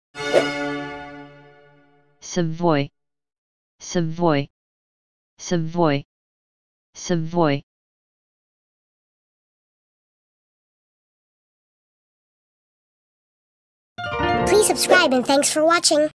Savoie (How to Pronounce Cities of the World)💬⭐🌍✅
This video is about "Savoie". This video series is something special. We're covering the pronunciation of all the cities of the world.